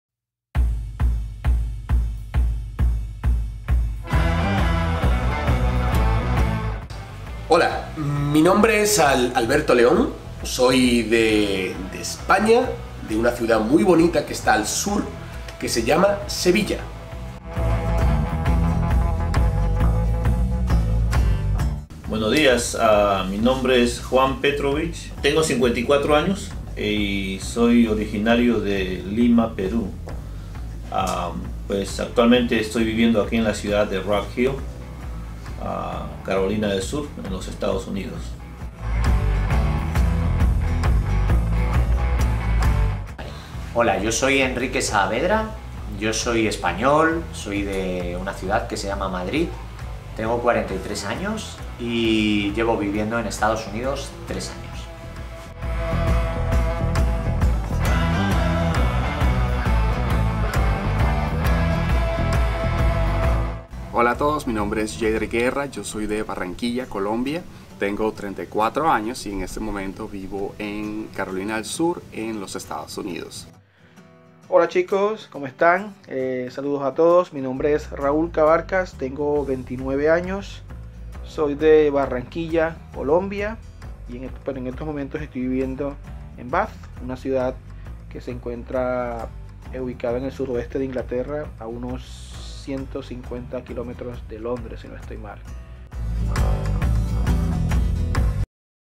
¿De Dónde Eres?
4 men from different Spanish speaking countries say their names, ages, where they are from and where they live. Watch the video and test your listening comprehension skills with the following quiz:

4 hombres de países hispanos hablantes dicen sus nombres, edades, sus nacionalidades y donde viven. Mira el video y responde las preguntas en el siguiente enlace: